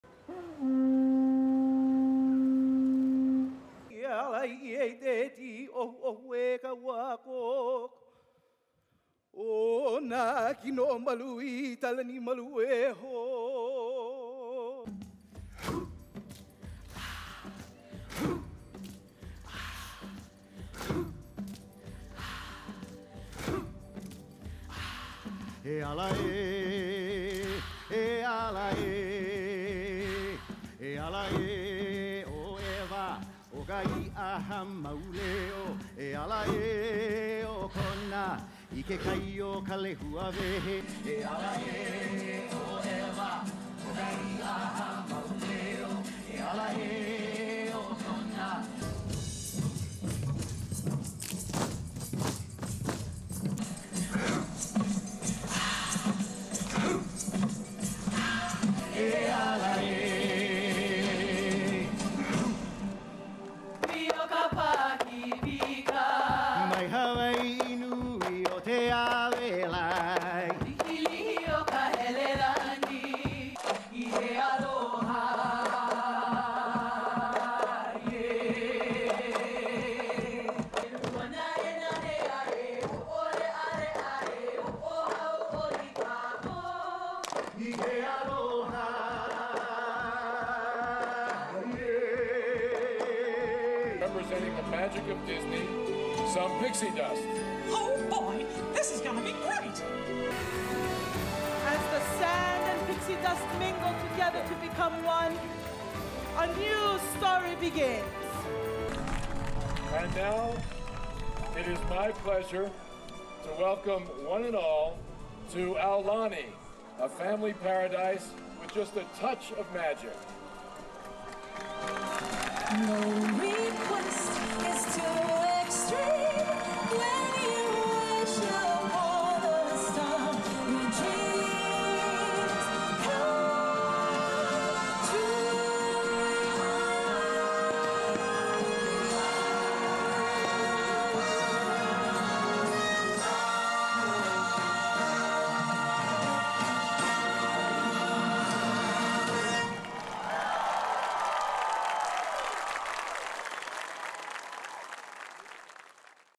Aulani grand opening ceremony for new Disney resort hotel in Hawaii
for more from Aulani, a Disney Resort & Spa in Hawaii!

On the evening of September 22, 2011, Disney celebrated the official grand opening of their new resort hotel Aulani in Ko Olina Hawaii with a special ceremony filled with Hawaiian tradition.